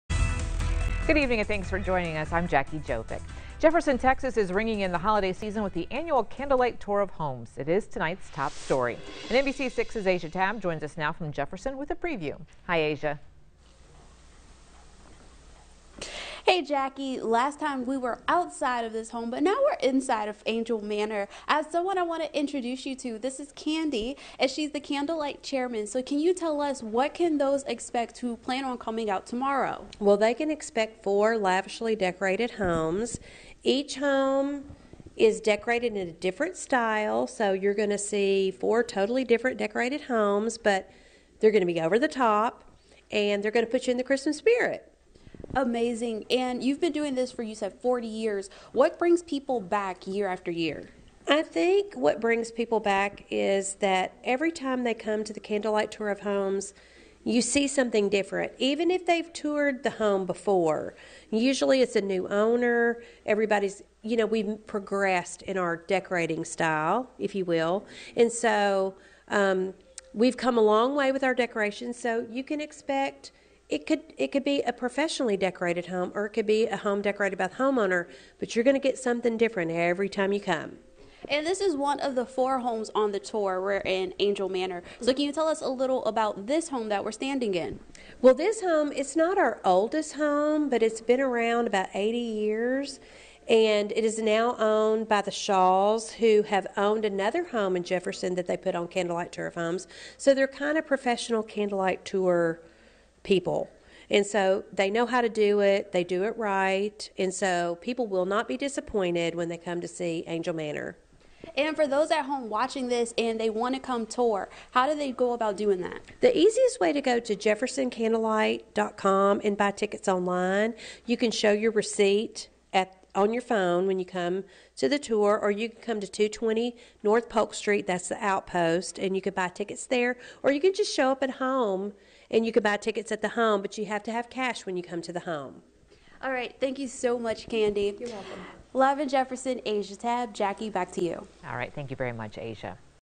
Jefferson Candlelight Tour of Homes returns
The anticipated 40th Annual Candlelight Tour of Homes made its return to Jefferson.